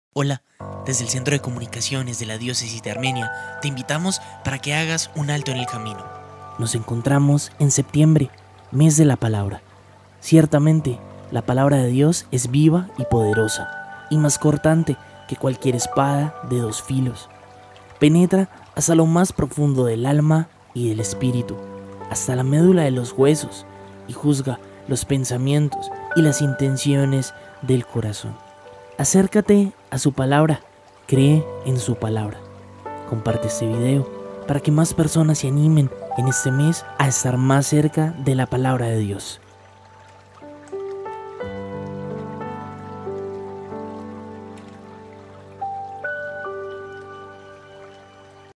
UN ALTO EN EL CAMINO | CREE EN SU PALABRA | DIÓCESIS DE ARMENIA
AltoEnElCamino 🛑 | A esta hora, el Centro de Comunicaciones de la Diócesis de Armenia, te invita a hacer un ALTO EN EL CAMINO, para que contemples el mejor regalo que Dios ha dejado a los hombres, su Palabra. Y en este mes que es dedicado a la Biblia es el momento propicio para profundizar en la Palabra de Dios 📖🙏🏻

👉🏻 Recuerda compartir este video con aquellas personas que crees que lo necesitan